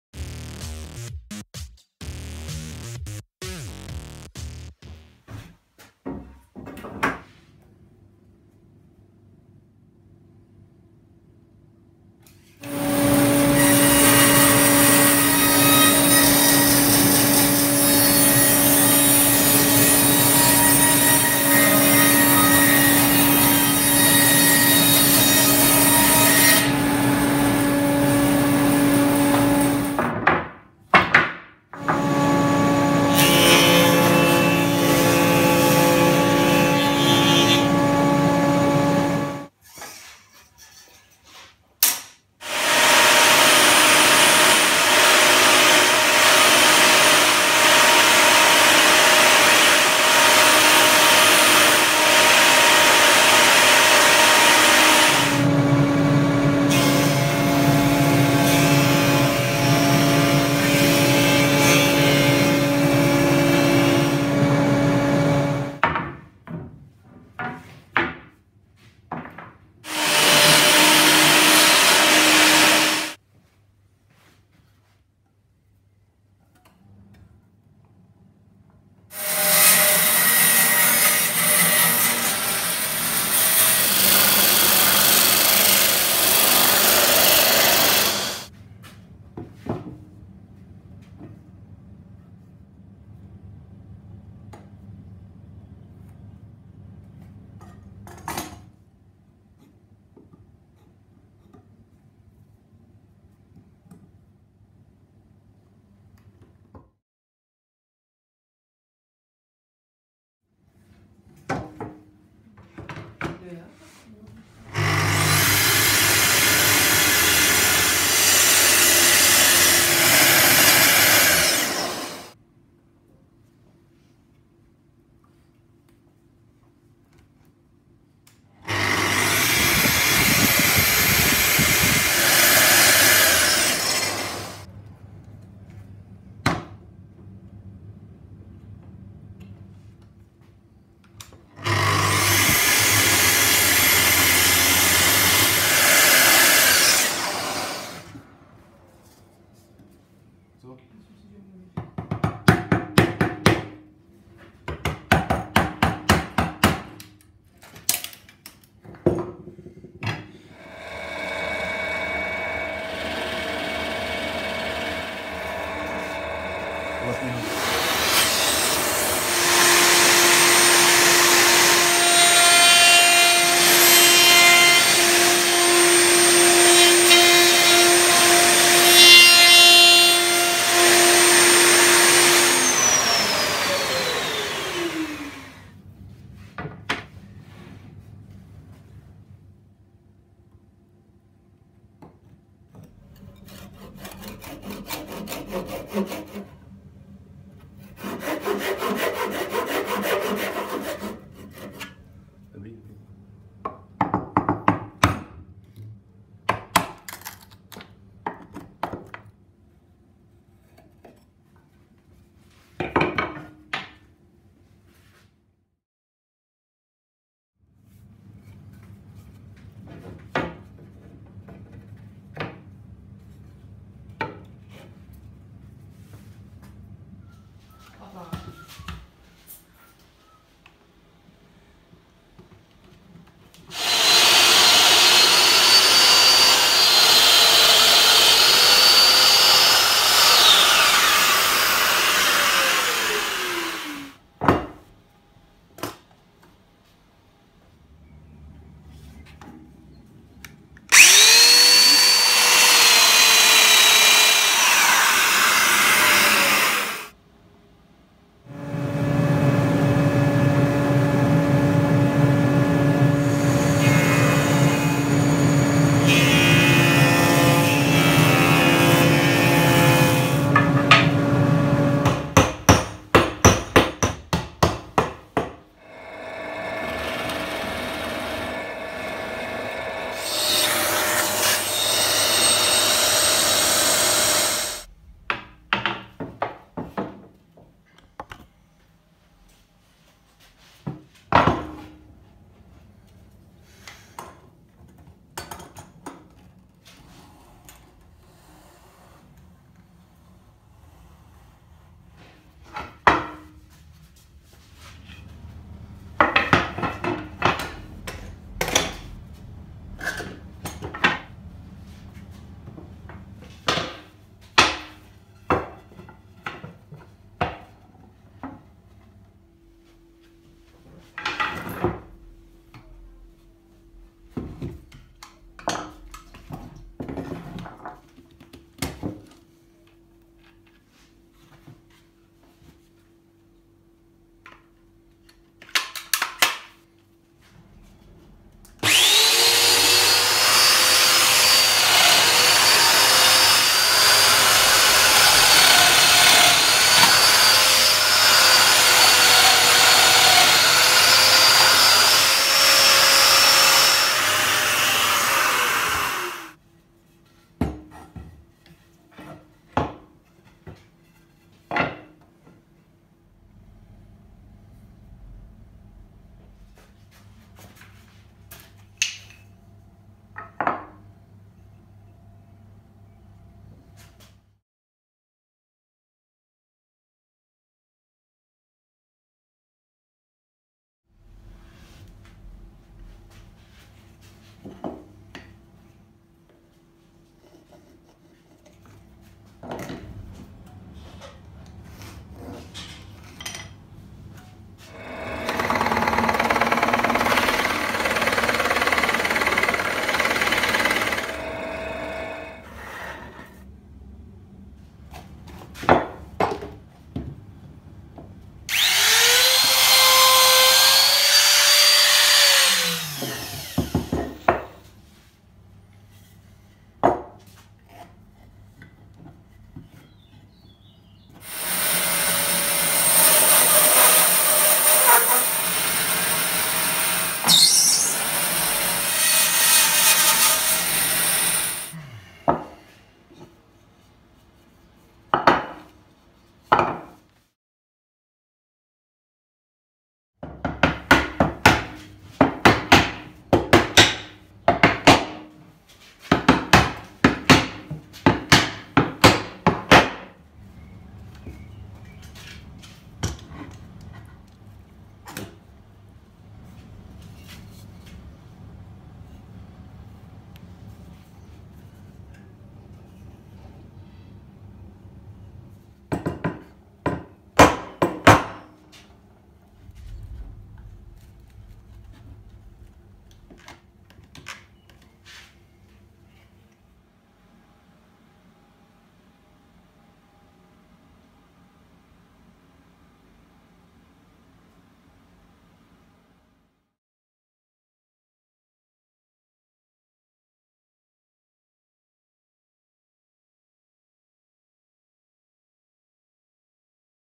Malířský stojan / 1. část / Painting easel 1. part
Moje dcera ráda maluje, tak jsem se rozhodl, že jí vyrobím maliřský stojan.
Přeci to nebudu kupovat, když to můžu vyrobit.